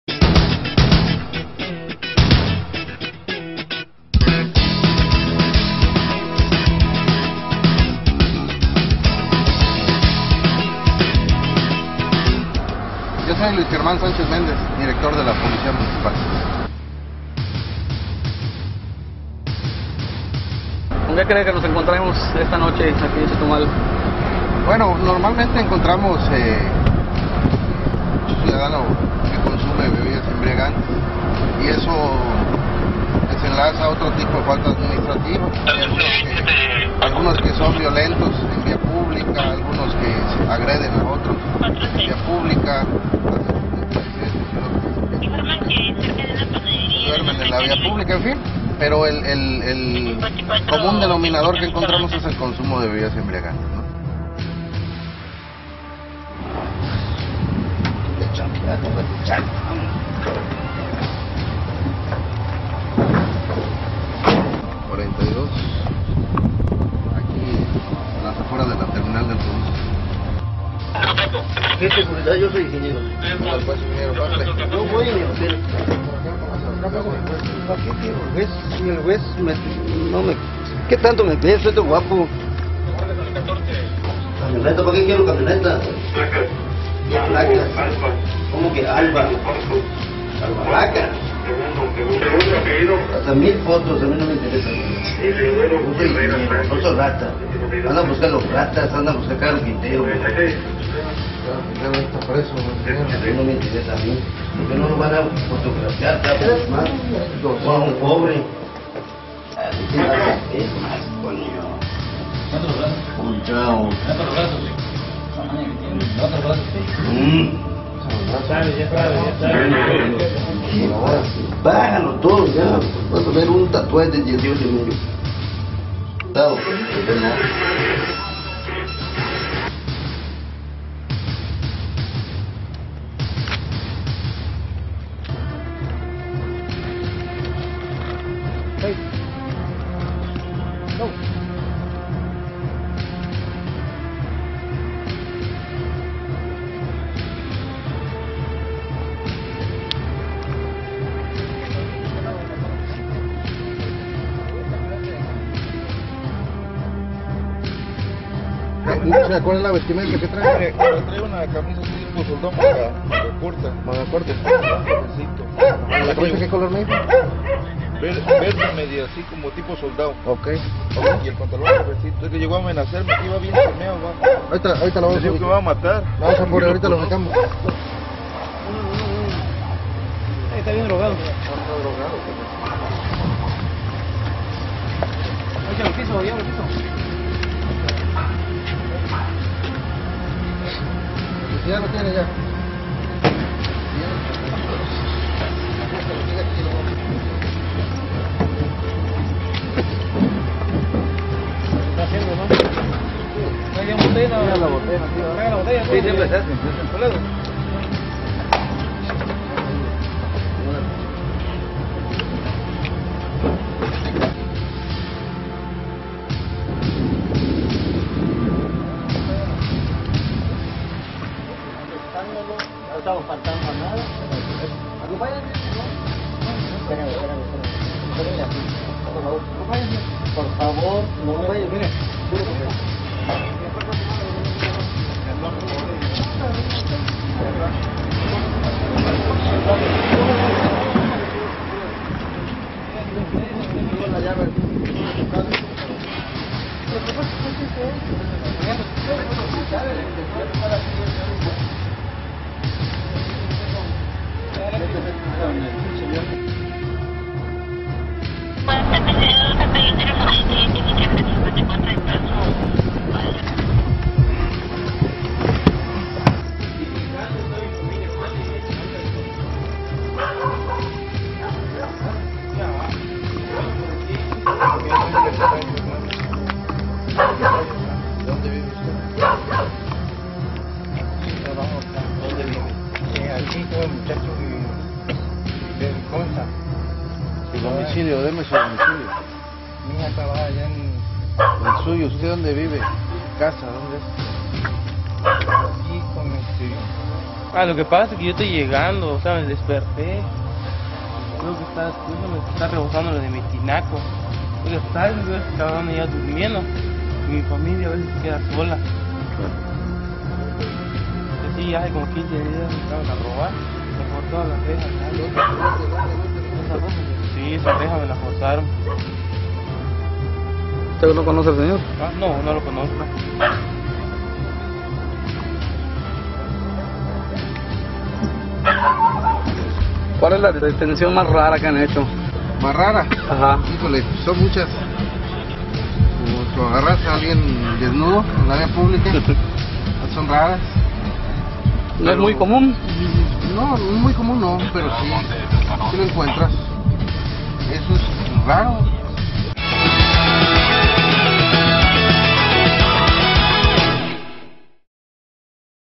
Policiaca Chetumal parte I
1ra parte recorrido en patrulla de policia por Chetumal